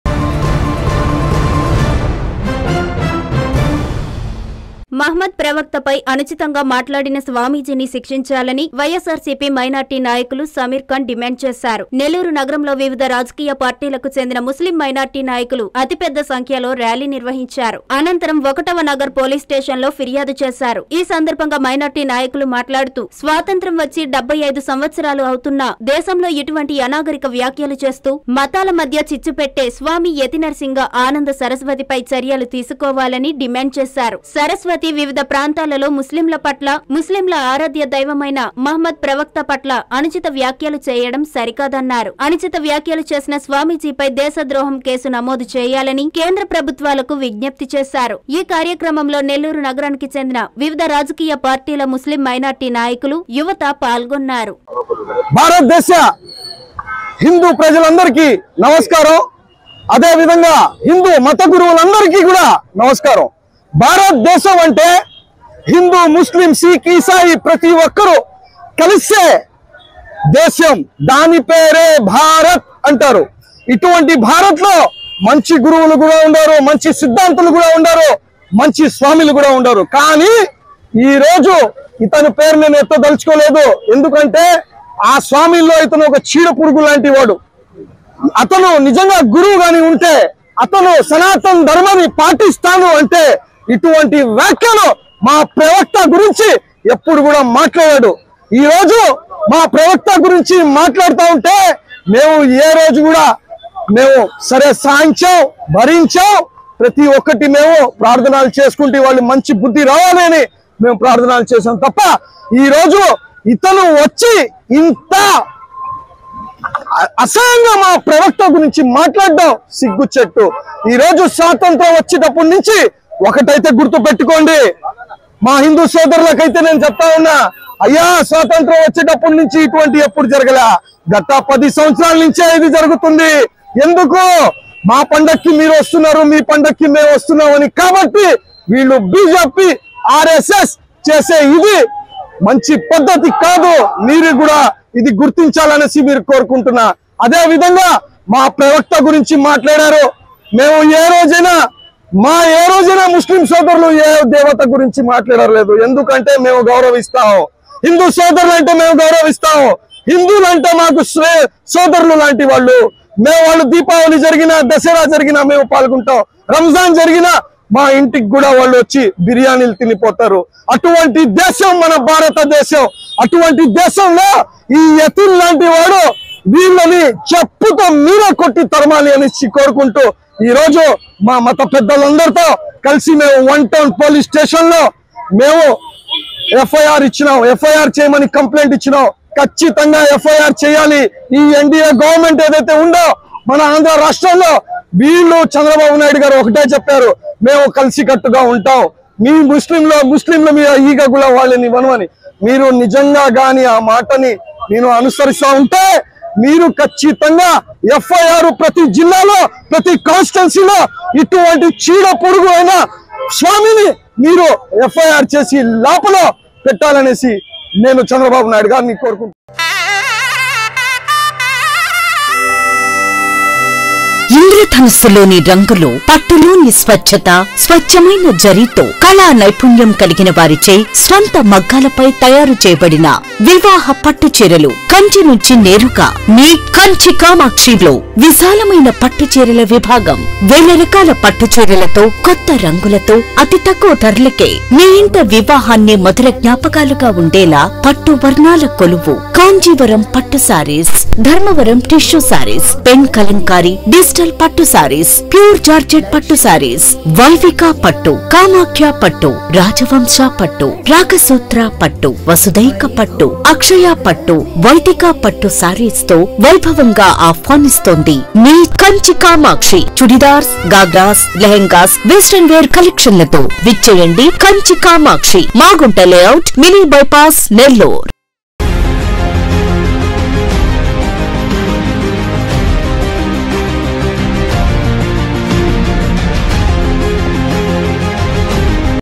ఆ స్వామిజీని శిక్షించండి
- మహమ్మద్ ప్రవక్త పై అనుచిత వ్యాఖ్యలు సరికాదు
- మత విద్వేషాలు రెచ్చగొట్టేందుకు కొందరి కుట్ర
- చర్యలు తీసుకోవాలని ఫిర్యాదు చేసిన మైనార్టీ నాయకులు సమీర్ ఖాన్